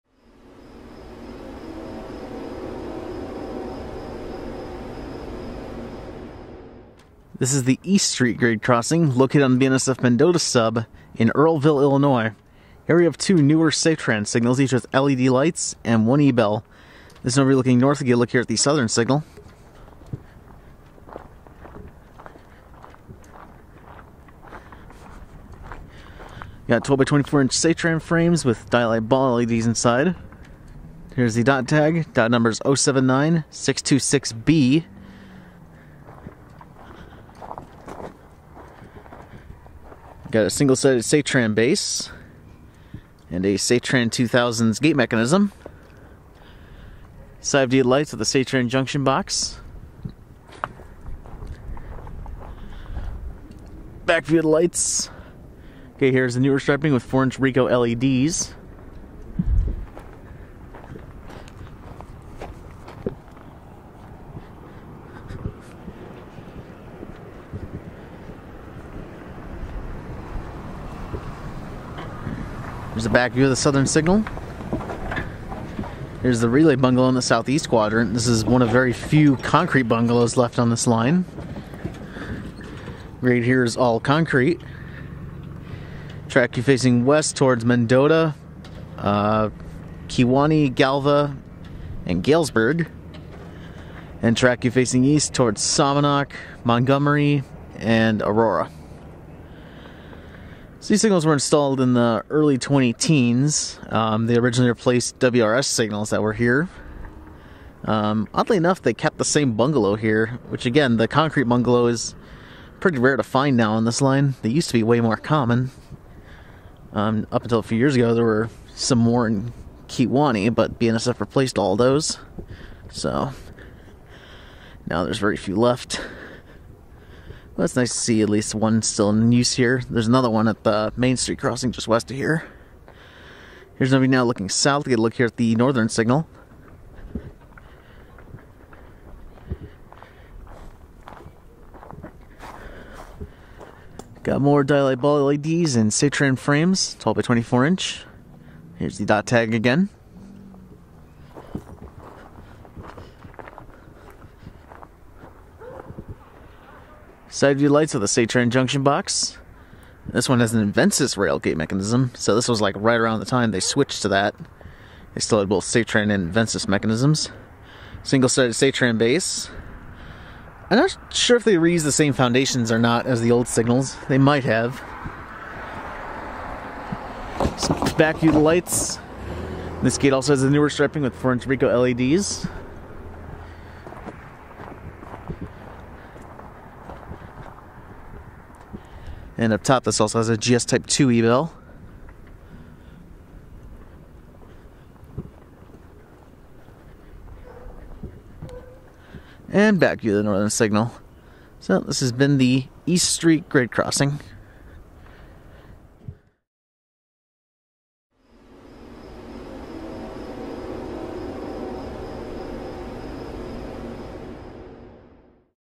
East St. Grade Crossing Tour Earlville, IL 12/9/21
This crossing is located on the BNSF Mendota Sub.  These signals were installed in the early 2010s replacing WRRS signals.

Northern Signal:
Bell:  GS Type 2 E Bell
Base:  Safetran Single Sided
LED Types:  Four Dialight Ball LEDs
Light Frames:  Four 12"x24" Safetran frames
Junction Box:  Safetran
Gate Lights:  4" RECO LEDs
Gate Mechanism:  Invensys Rail

Southern Signal:
Bell:  None